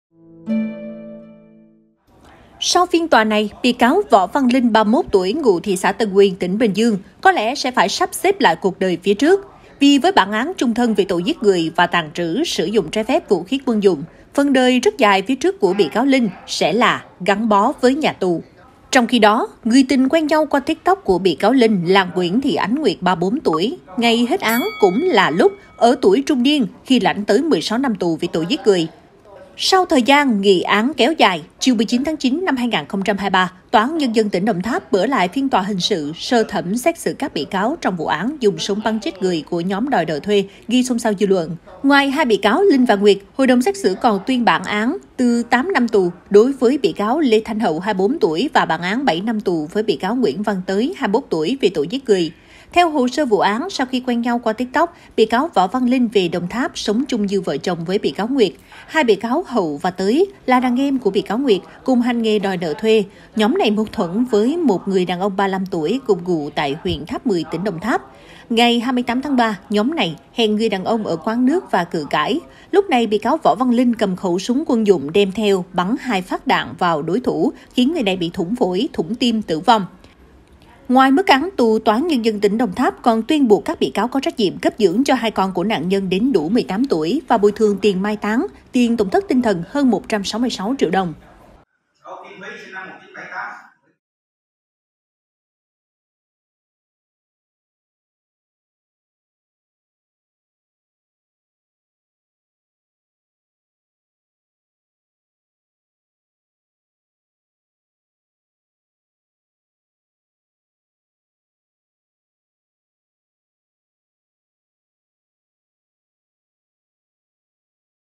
Tuyên án cặp tình nhân quen qua TikTok, đòi nợ thuê bắn chết người
Mâu thuẫn nhau trong quá trình đòi nợ thuê, nhóm bị cáo hành nghề đòi nợ thuê tại Đồng Tháp đã dùng súng bắn chết một người trong nhóm rồi bỏ trốn.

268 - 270 Nguyễn Đình Chiểu, Phường Võ Thị Sáu, Q.3, TP HCM